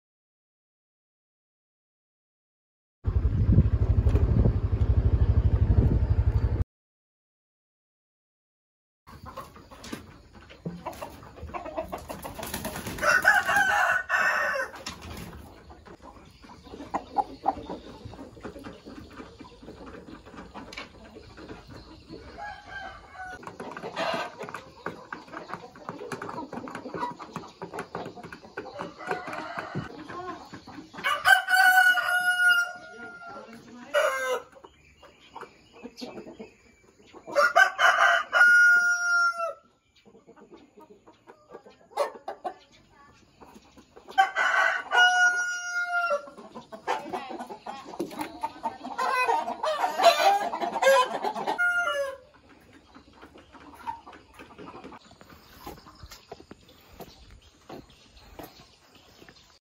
PART 3 MAGANG TEKNIS AYAM NUNUKAN OLEH MAHASISWA POLBANGTAN MALANG (PPKH5A)
Magang teknis oleh mahasiswa Polbangtan malang di DPKP AYAM NUNUKAN KALIMANTAN UTARA